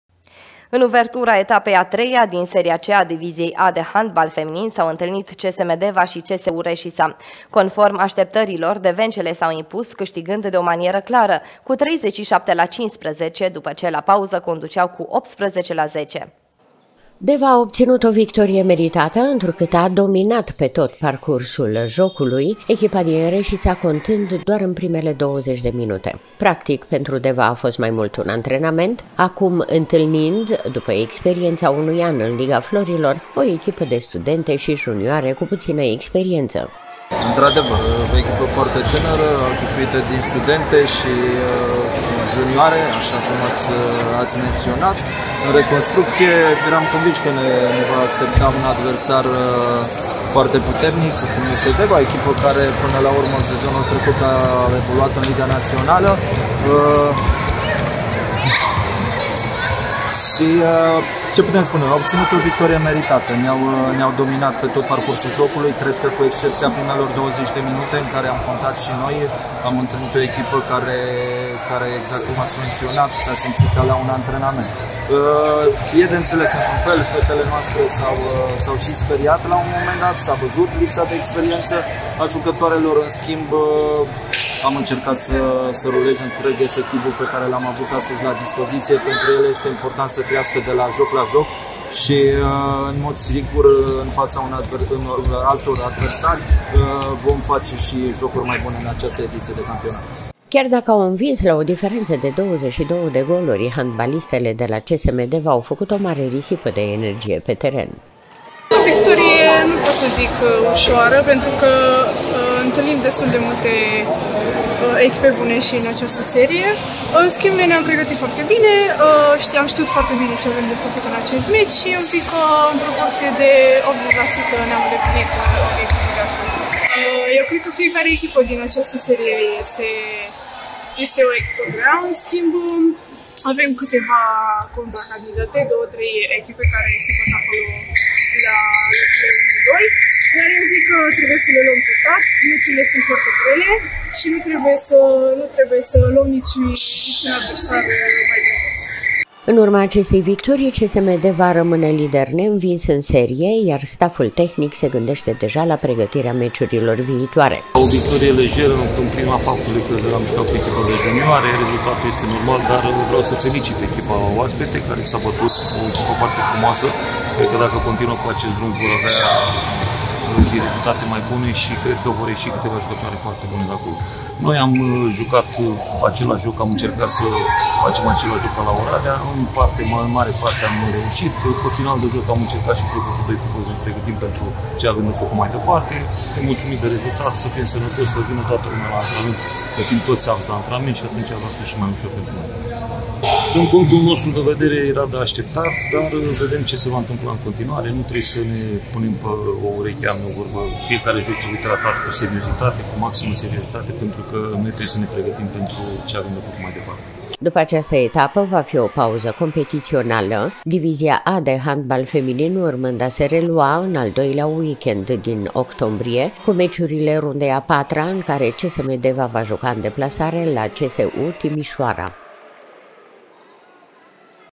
VICTORIE CATEGORICĂ PENTRU CSM DEVA ÎN DIVIZIA A DE HANDBAL FEMININ
În uvertura etapei a treia din seria C a Diviziei A de handbal feminin s-au întâlnit CSM Deva și CSU Reșița. Conform așteptărilor, devencele s-au impus, câștigând de o manieră clară, cu 37-15, după ce la pauză conduceau cu 18-10.